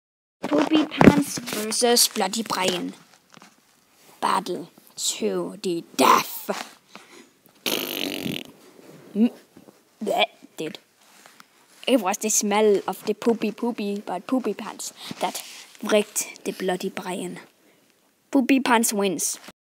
PoopyPants VS BloodyBrian!
My friend and i made a kind of Collab where he made PoopyPants and i made BloodyBrian! Well it's not very much but it's funny as hell!XD Oh and BTW BloodyBrian is my new OC now!:D I was voicing everything tho! I Think my friend is a bit shy at things like that!XD Bye!X0